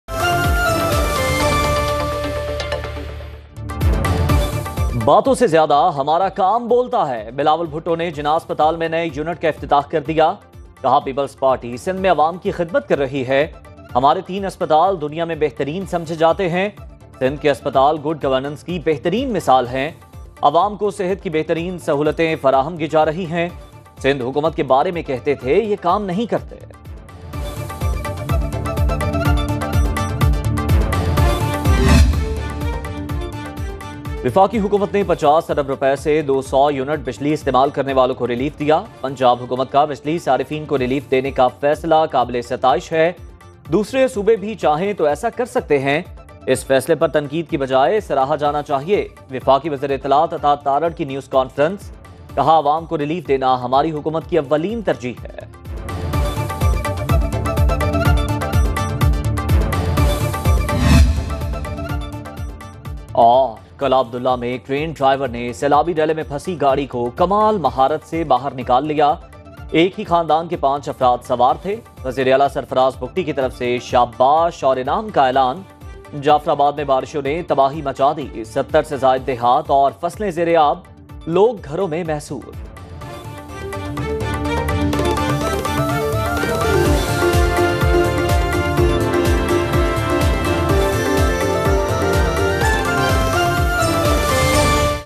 Rains Wreaked Havoc, High Alert | 5 PM News Headlines | 19 Aug 2024 | Suno News HD
Severe rains have caused widespread destruction across multiple regions in Pakistan, leading to floods and infrastructural damage. Authorities have issued a high alert and are urging residents to stay indoors and avoid unnecessary travel. The situation remains critical as rescue operations are underway to assist those affected.

Stay updated with the latest news and safety advisories only on Suno News HD.

📺 Watch Now on Suno News HD